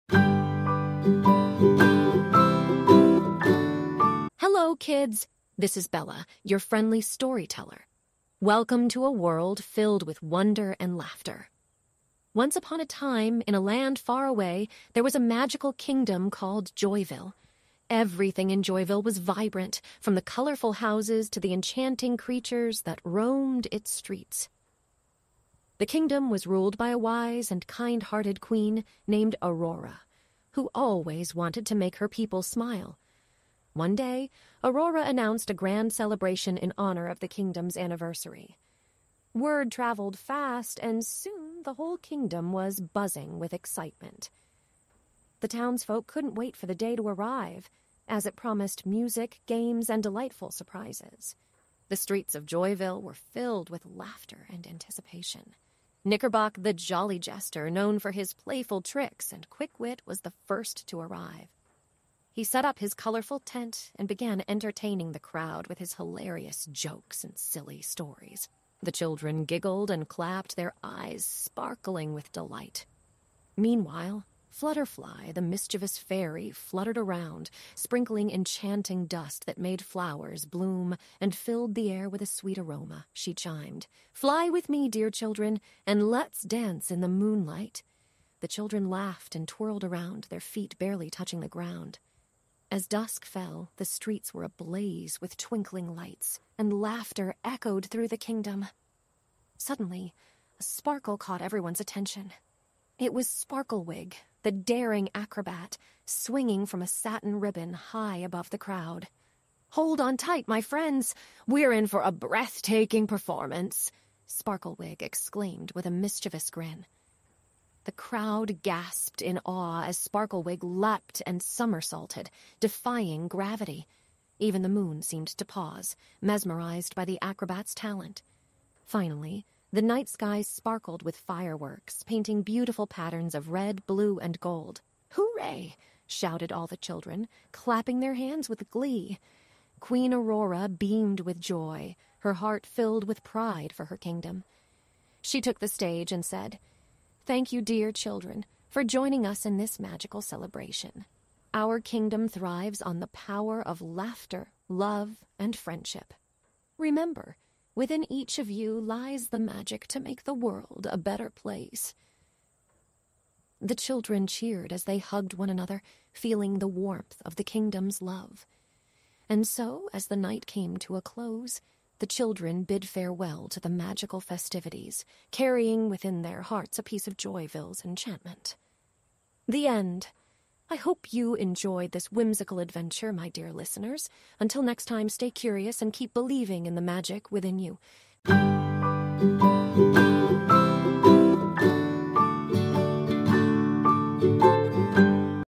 Bella's Whimsical Adventure | Bedtime Story for Kids
Join Bella, the friendly storyteller, in an enchanting adventure full of laughter and wonder. In the magical kingdom of Joyville, lively characters like Knickerbock, Flutterfly, and Sparklewig bring joy to children's hearts. Come along and immerse yourself in a world of vibrant colors, playful tricks, and breathtaking acrobatics. Let your imagination soar as Bella weaves a whimsical tale that will capture your imagination and leave you with a smile. Perfect for bedtime or any time!